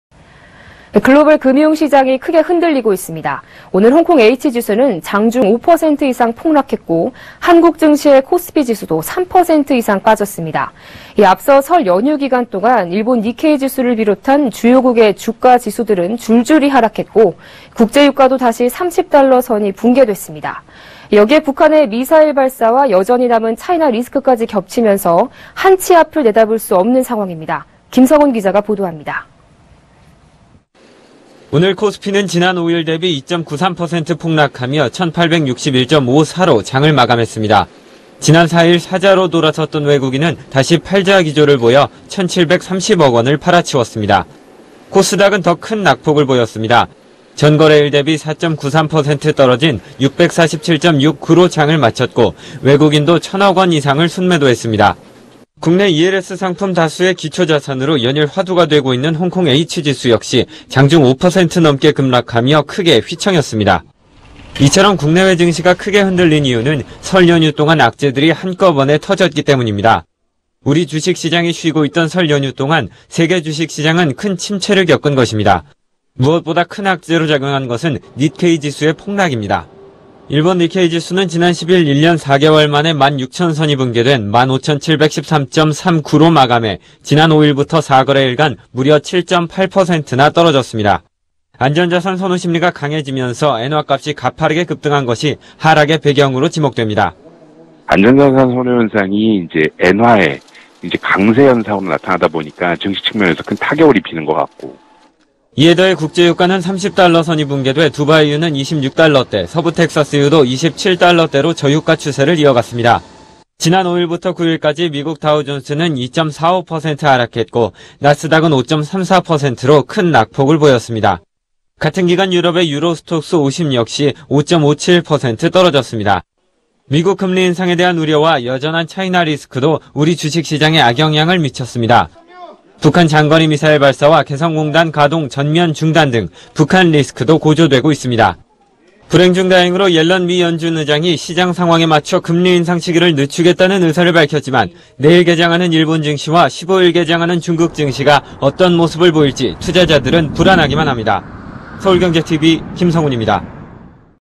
설연휴 쌓인 악재에 코스피 3% 급락
[앵커]
글로벌금융시장이 크게 흔들리고 있습니다. 오늘 홍콩H지수는 장중 5%이상 폭락했고, 한국증시의 코스피지수도 3%이상 빠졌습니다. 앞서 설연휴기간동안 일본 닛케이지수를 비롯한 주요국 주가지수들은 줄줄이 하락했고, 국제유가도 다시 30달러 선이 붕괴됐습니다. 여기에 북한의 미사일 발사와 여전히 남은 차이나 리스크까지 겹쳐 한치 앞을 내다볼 수 없는 상황입니다. 김성훈기자가 보도합니다.

[기자]
오늘 코스피는 지난 5일 대비 2.93% 폭락하며 1,861.54로 장을 마감했습니다. 지난 4일 ‘사자’로 돌아섰던 외국인은 다시 팔자 기조를 보여 1,730억원을 팔아치웠습니다.

코스닥은 더 큰 낙폭을 보였습니다. 전 거래일 대비 4.93% 떨어진 647.69로 장을 마쳤고, 외국인도 1,000억원 이상을 순매도했습니다. 국내 ELS 상품 다수의 기초자산으로 연일 화두가 되고 있는 홍콩 H지수 역시 장중 5% 가까이 급락하며 크게 휘청였습니다.

이처럼 국내외 증시가 크게 흔들린 이유는 설 연휴동안 악재들이 한꺼번에 터졌기 때문입니다. 우리 주식시장이 쉬고 있던 설 연휴 동안 세계 주식시장은 큰 침체를 겪은 것입니다. 무엇보다 큰 악재로 작용한 것은 닛케이지수의 폭락입니다. 일본 닛케이지수는 지난 10일 1년4개월 만에 1만6000선이 붕괴된 1만5713.39로 마감해 지난 5일부터 4거래일간 무려 7.8%나 떨어졌습니다. 안전자산 선호 심리가 강해지면서 엔화값이 가파르게 급등한 것이 하락의 배경으로 지목됩니다.

[녹취] 김용구 / 삼성증권 리서치센터 수석연구원
안전자산 선호현상이 엔화 강세현상으로 나타나다 보니 증시 측면에서 큰 타격을 입히는 것 같고...

이에더해 국제 유가는 30달러 선이 붕괴되 두바이유는 26달러대, 서부텍사스유도 27달러대로 저유가 추세를 이어갔습니다. 지난 5일부터 9일까지 미국 다우존스는 2.45% 하락했고, 나스닥은 5.34%로 큰 낙폭을 보였습니다. 같은 기간 유럽의 유로스톡스50 역시 5.57% 떨어졌습니다.

미국 금리 인상에 대한 우려와 여전한 차이나 리스크도 우리 주식시장에 악영향을 미쳤습니다. 북한 장거리미사일 발사와 개성공단 가동 전면중단등 북한리스크도 고조되고 있습니다.

불행 중 다행으로 옐런 미 연준의장이 시장 상황에 맞춰 금리인상 시기를 늦추겠다는 의사를 밝혔지만, 내일 개장하는 일본 증시와 15일 개장하는 중국증시가 어떤 모습을 보일지 투자자들은 불안하기만 합니다. / 서울경제TV 김성훈입니다.